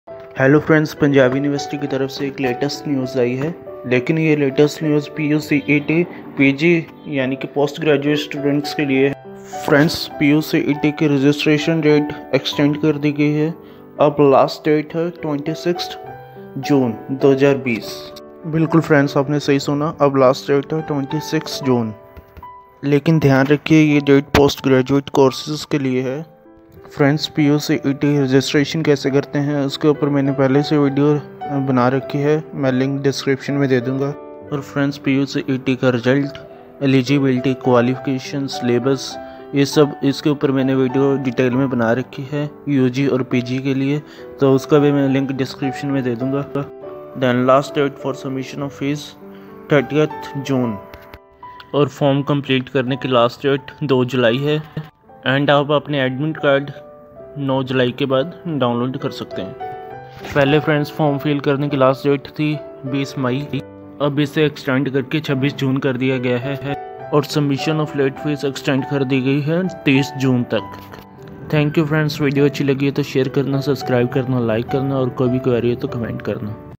PU CET (PG) 2020 – Dates (Extended) || Fee || Completion of Form || Admit Card
PU CET (PG) 2020 – Dates (Extended)

Hy Guys. !!

I Appreciate for Your HELP & giving me some TIME.
THANKS.